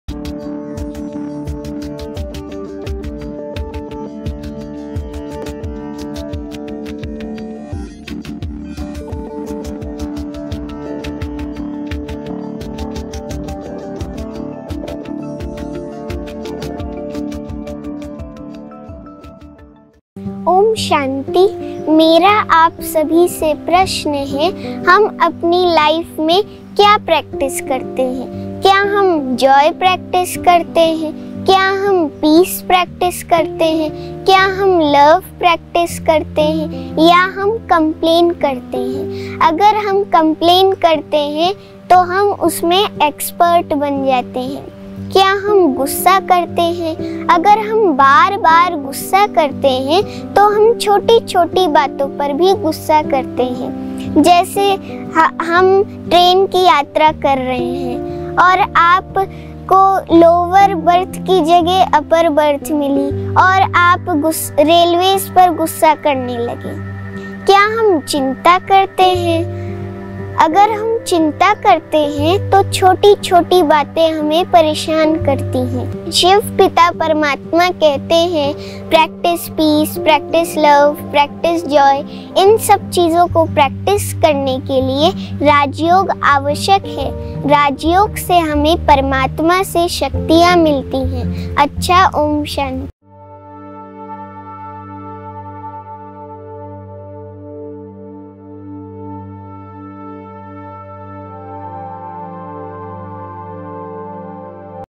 LITTLE ANGELS - MEDITATE FOR PEACE & LOVE BY BHUVI
Bhuvi says - What do we practice in our lives? Do we practice Joy?  Do we practice Peace?  Do we practice Love?  or do we complain?
If we complain, then we become  an expert in it.  Do we get angry?
If we get angry repetedly, thenwe start getting angry for little thi ngs too. How She vividly explained through an example? Watch it now.